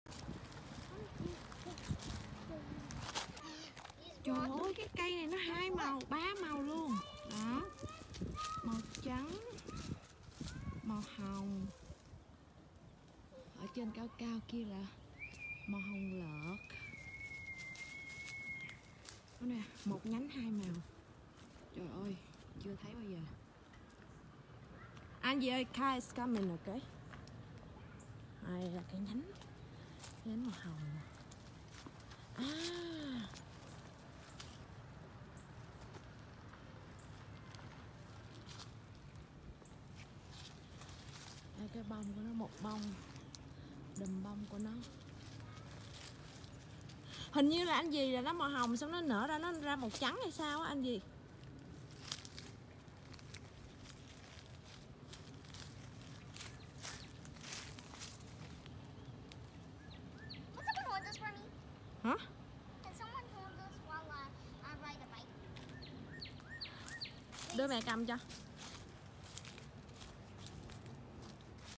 Hoa đào nở một cây 3 màu đẹp lạ chưa từng thấy / Flowering peppermint peach
Hoa đào nở vào mùa xuân đã đẹp. Nhưng đây là  lần  tiên thấy một cây ba màu vừa đẹp vừa độc đáo.